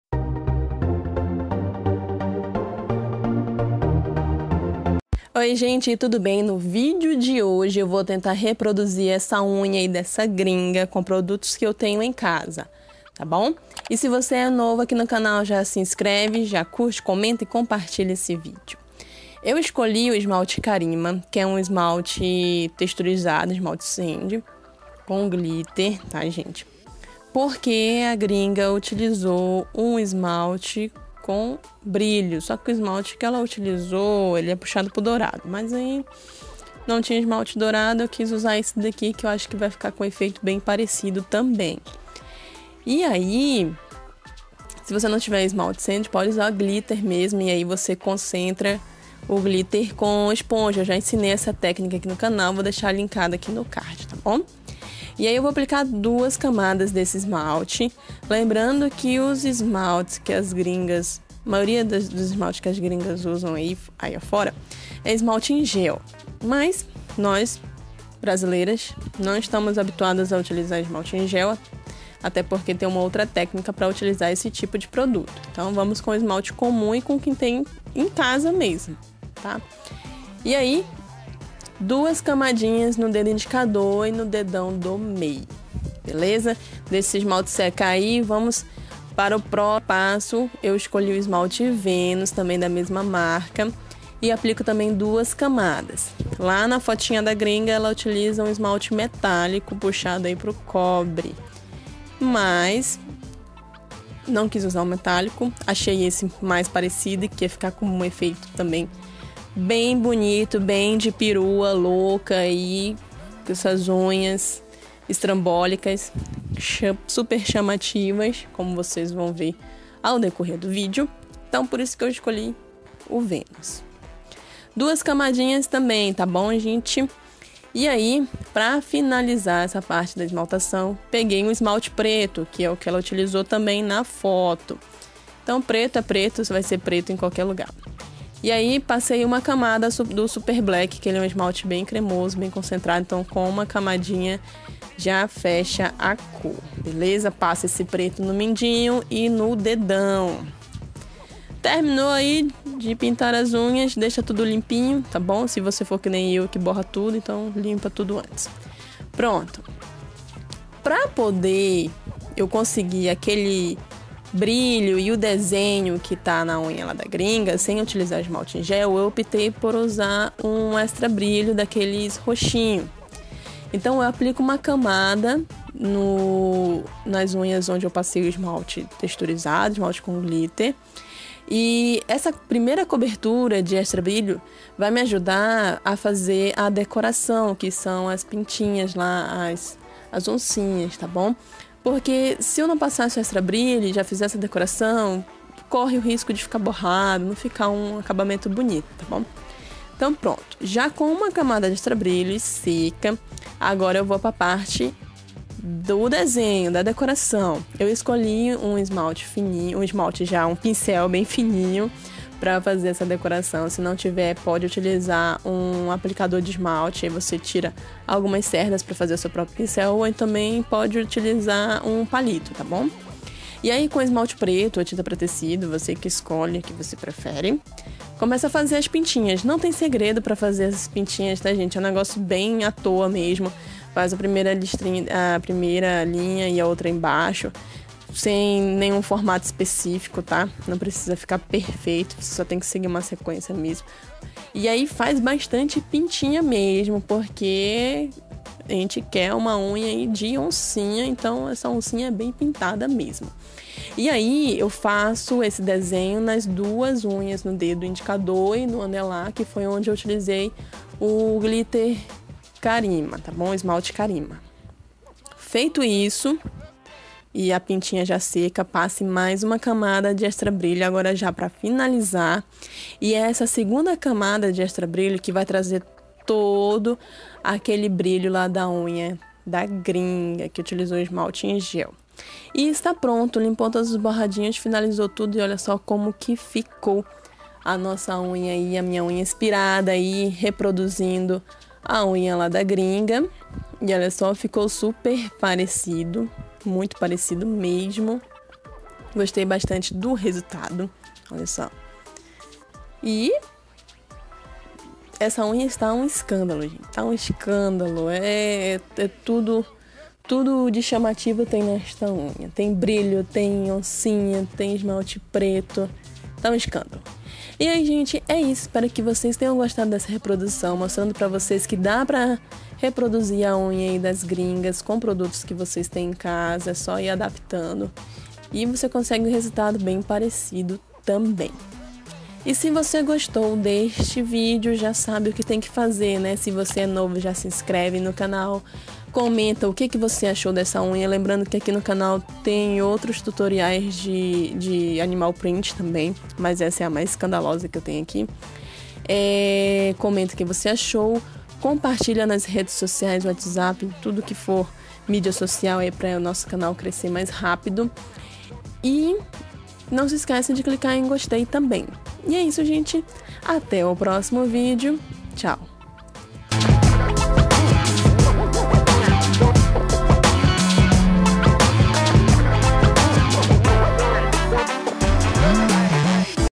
UNHAS DECORADAS SUPER FÁCIL | IMITANDO UNHAS DO INSTAGRAM
Para o vídeo de hoje, eu trouxe mais um Expectativa x Realidade que tá babado. Escolhi uma unha que está super fácil para você adaptar em casa, e o melhor, com produtos de fácil acesso. Tá pensando que é só as gringas que conseguem?? Vem comigo e confira o passo a passo dessa Nail art Animal Print digna de Perua.

Se inspirou em alguma decoração feita no canal? Então post no instagram com a tag blognailfe.
Bjão